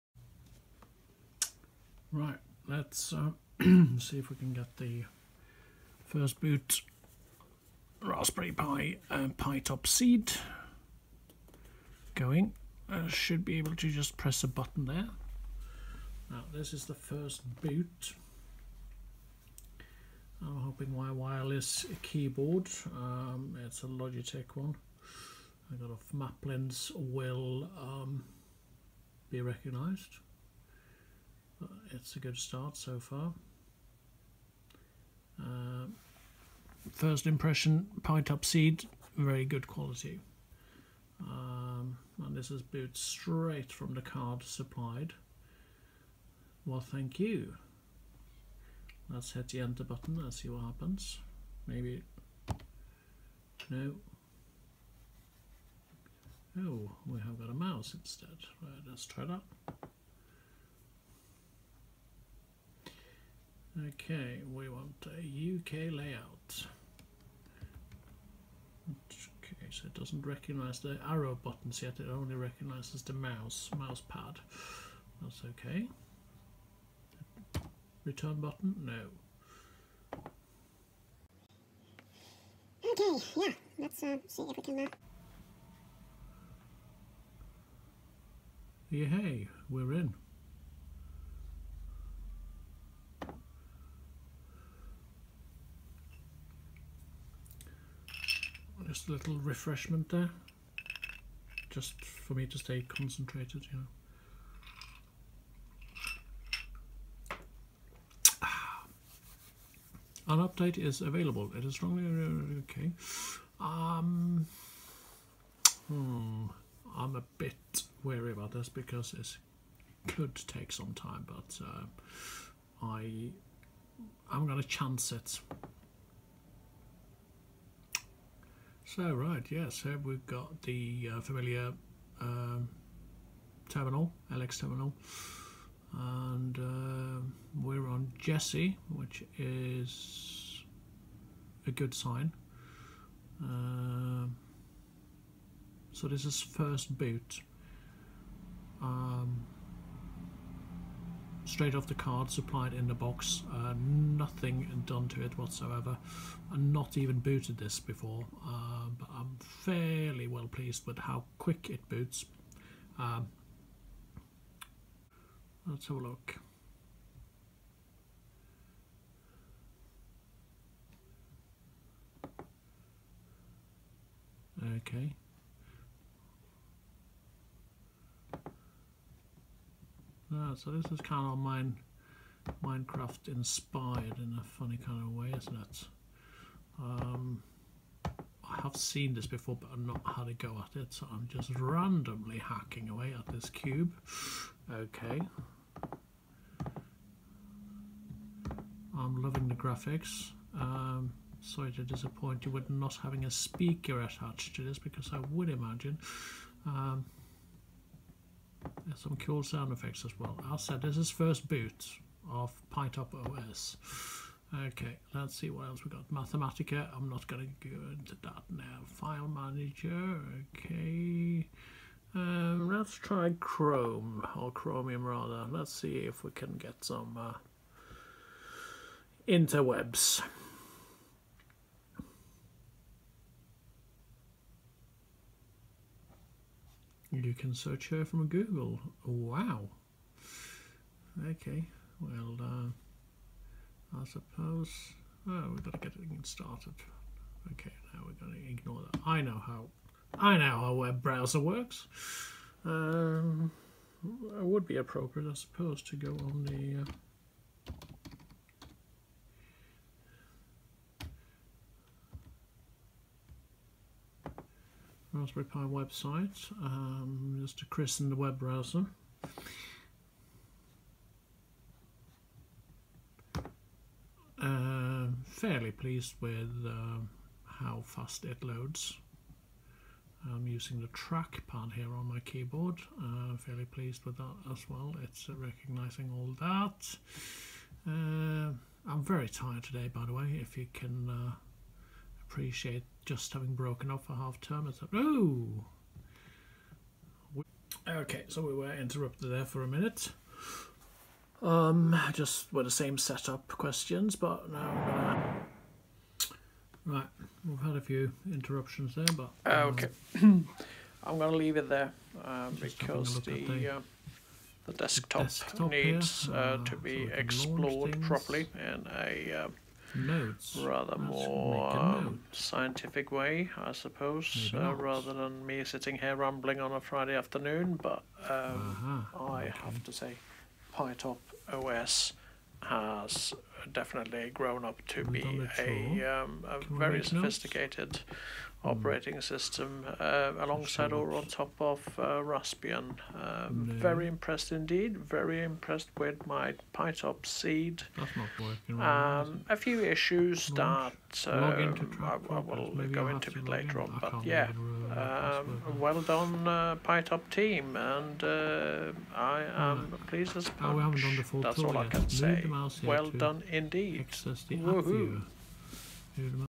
pi-top CEED first boot
A very casual , unofficial , lazy first boot of pi-top CEED ; very impressed so far !!